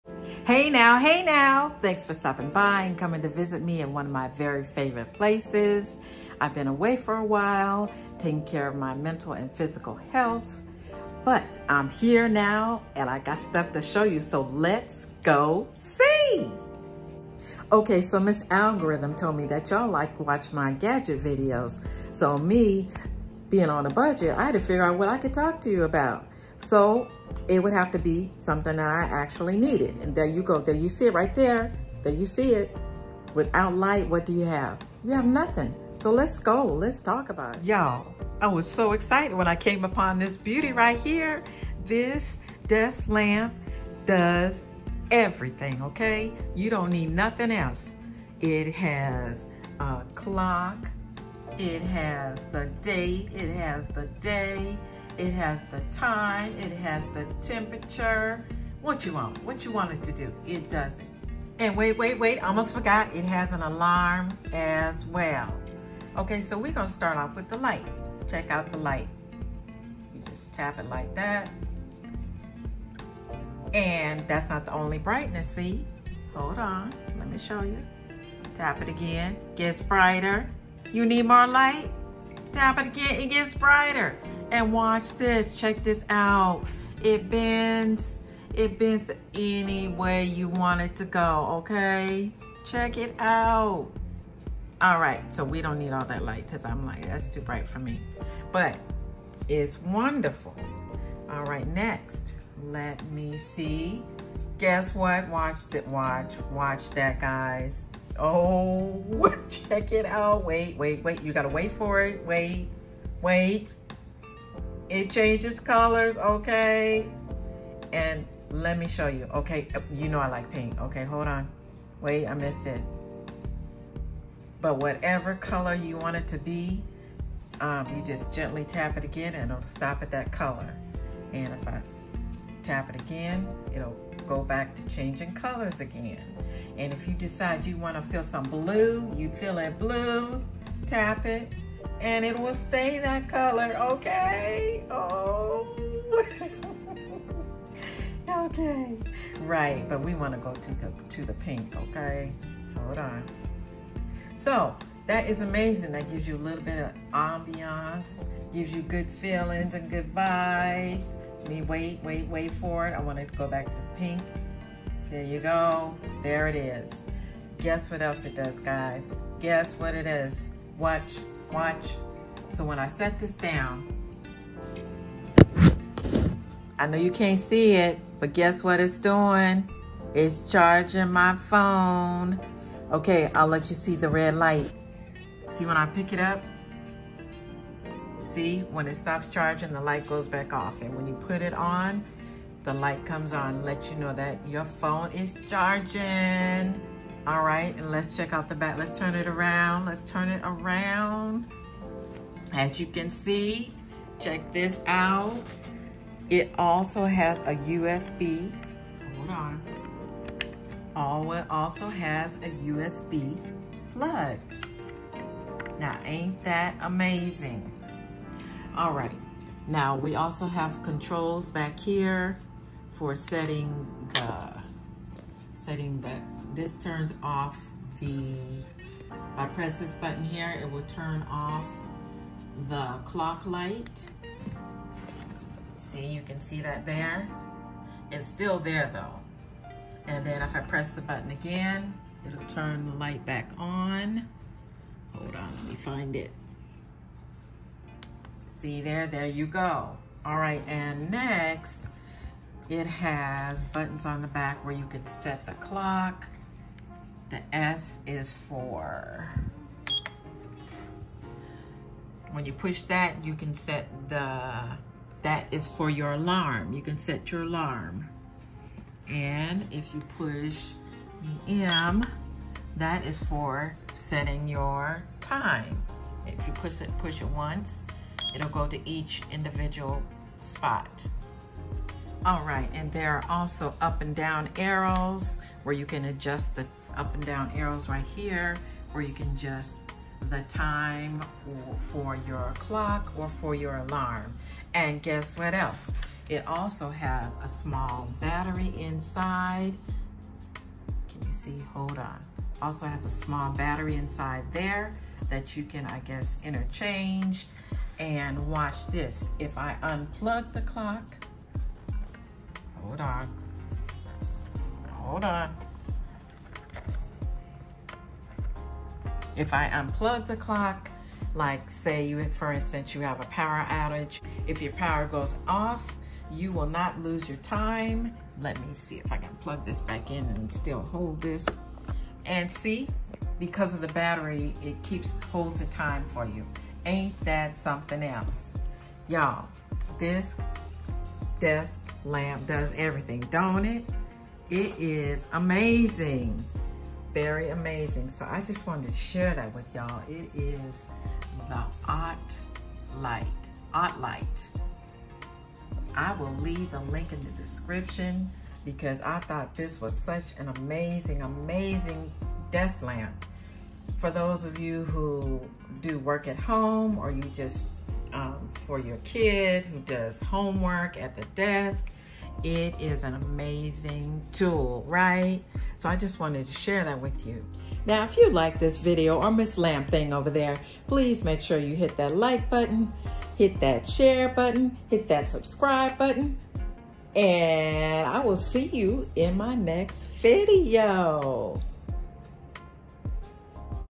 The Best Desk Lamp | Work From Home Accessories | Ottlite Wellness Series
Searching for the best desk lamp for your home office? Well, your search stops here, cause this Ottelite Wellness Edition Desk Lamp does EVERYTHING! Come on inside and see….oh….and bring a friend!!😉

Ottelite Wellness Series Desk Lamp